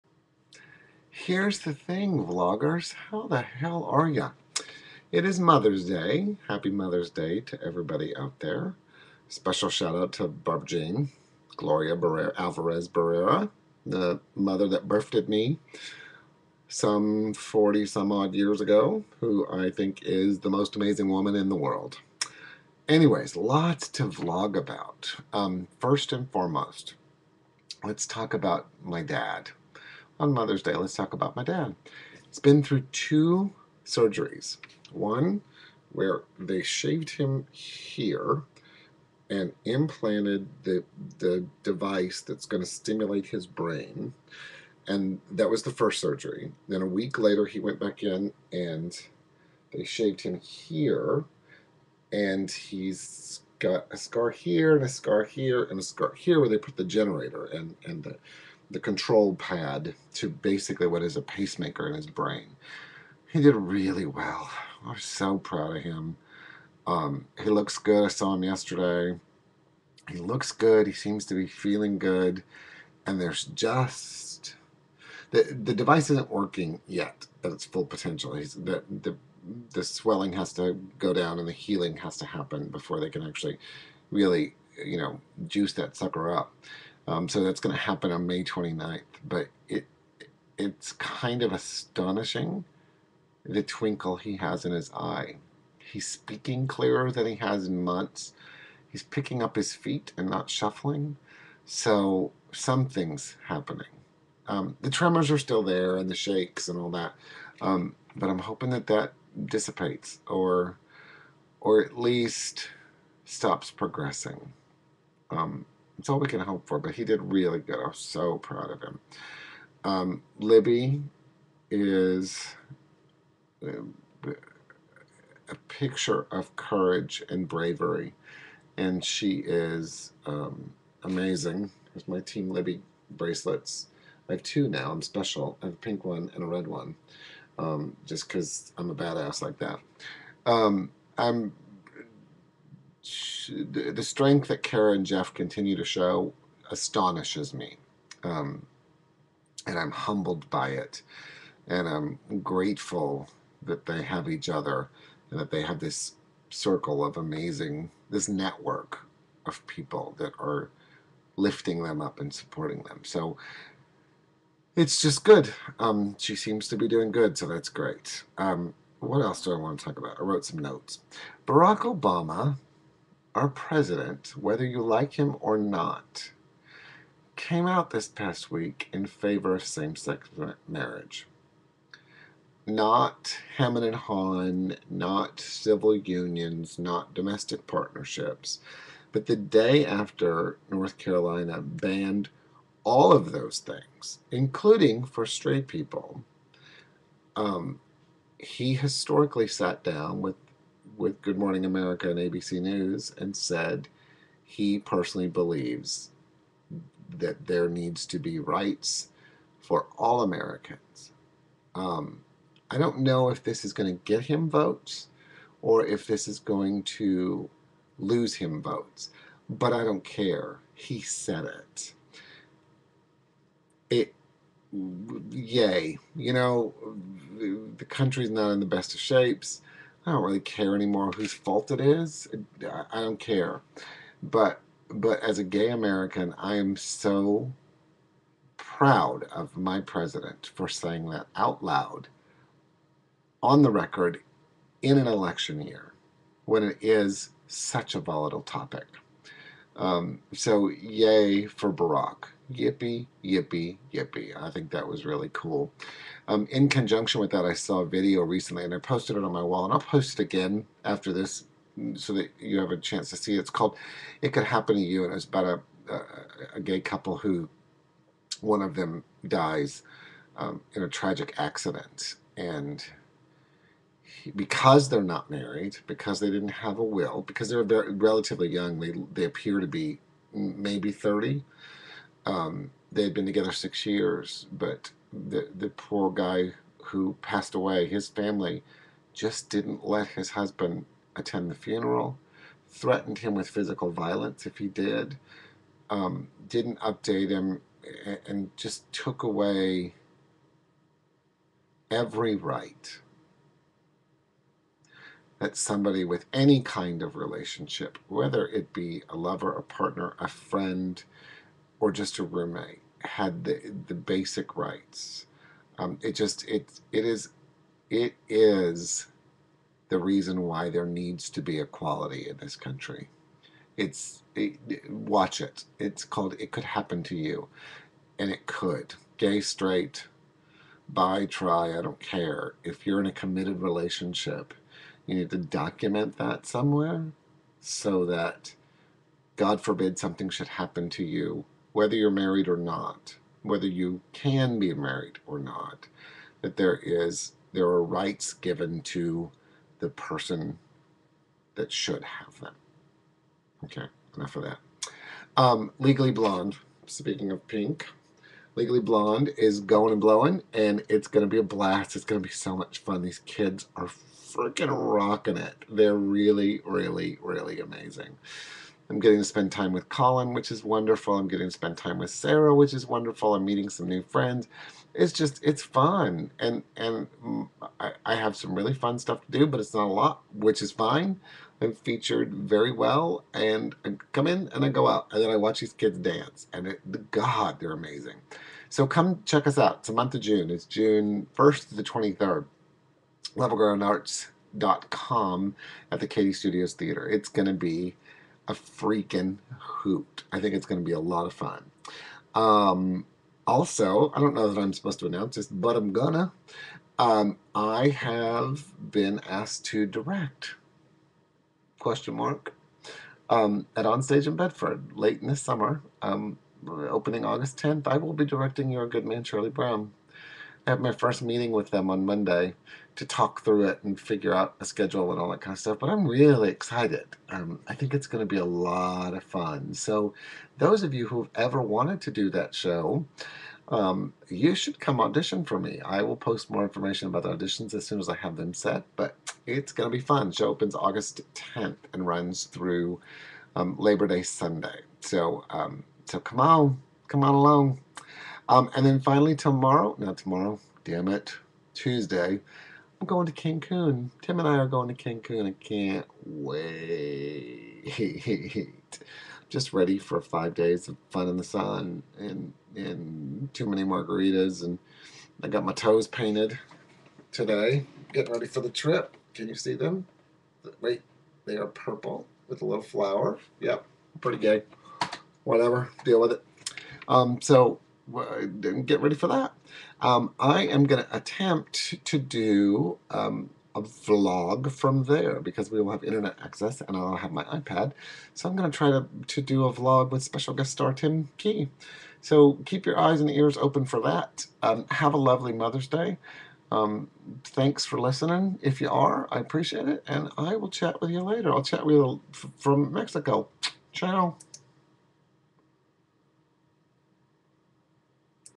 Here's The Thing -- Vlog 39
The President ROCKS -- Dad and Libby are doing great -- Shows that I'm doing and CANCUN